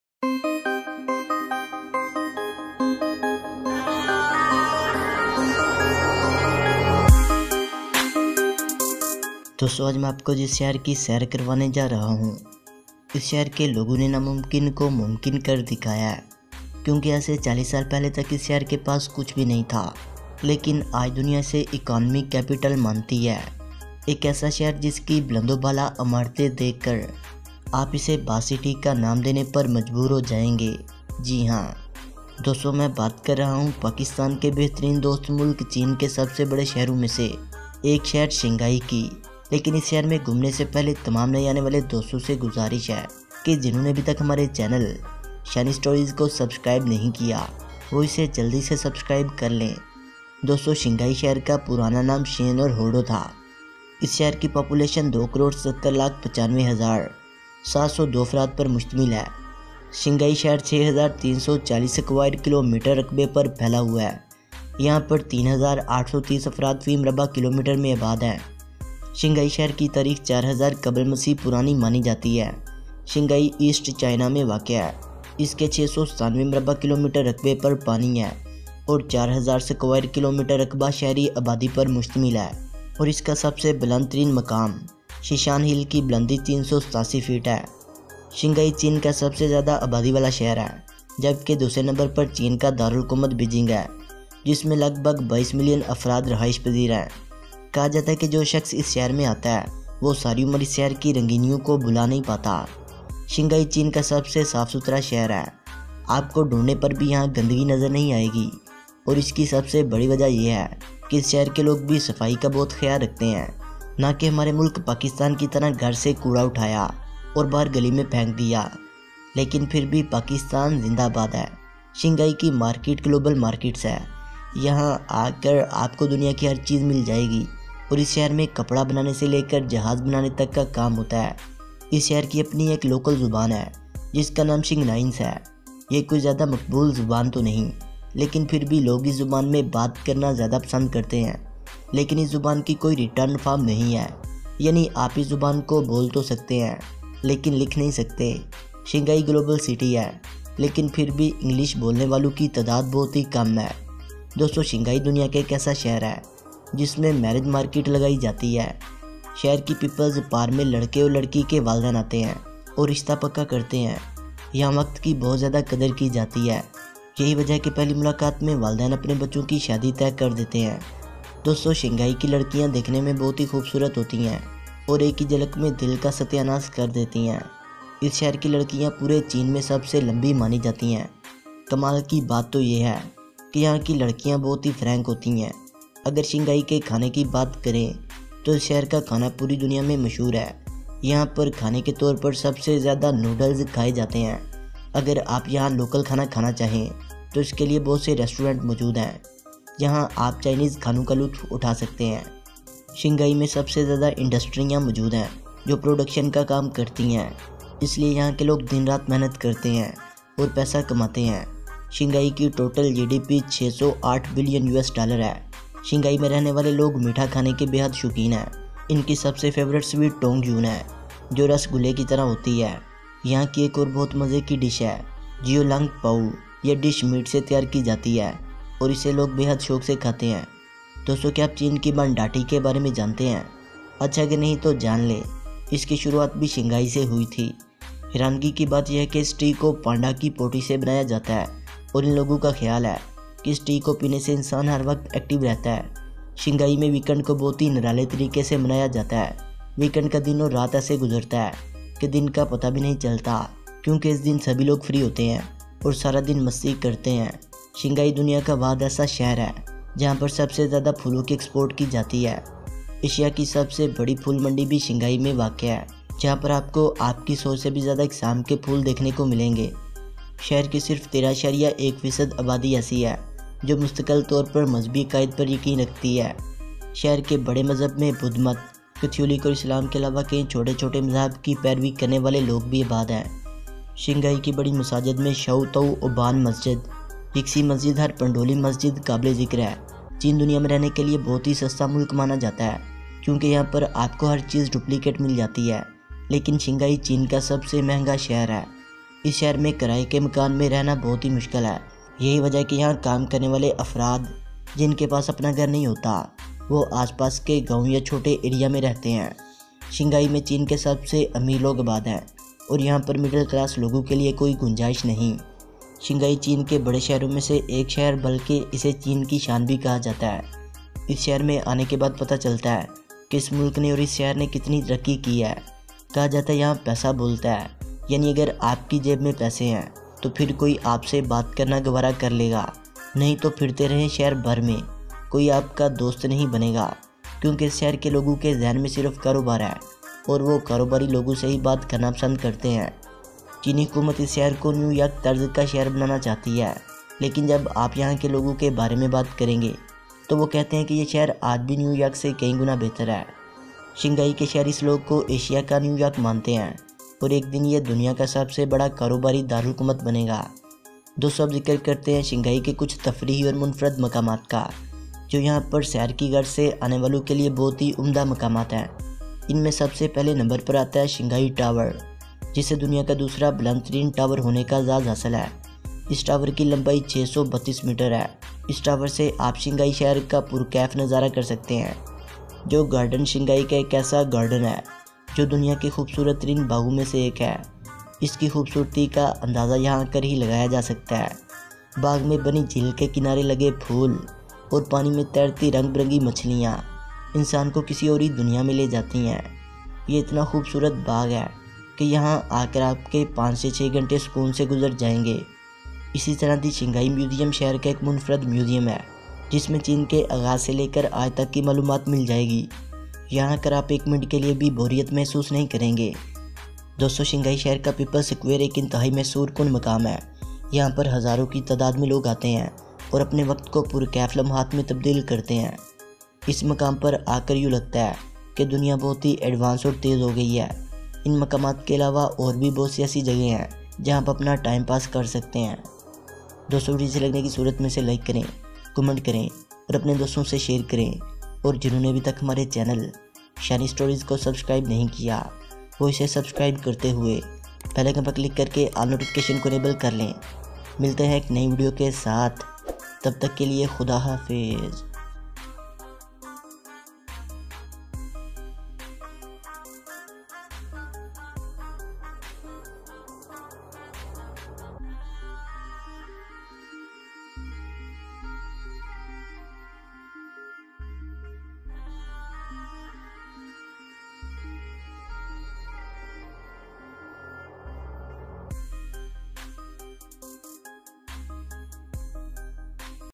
Facts About Shanghai | Travel To Shanghai China | Shani Stories
Shanghai, on China’s central coast, is the country's biggest city and a global financial hub. Its heart is the Bund, a famed waterfront promenade lined with colonial-era buildings. Across the Huangpu River rises the Pudong district’s futuristic skyline, including 632m Shanghai Tower and the Oriental Pearl TV Tower, with distinctive pink spheres. Sprawling Yu Garden has traditional pavilions, towers and ponds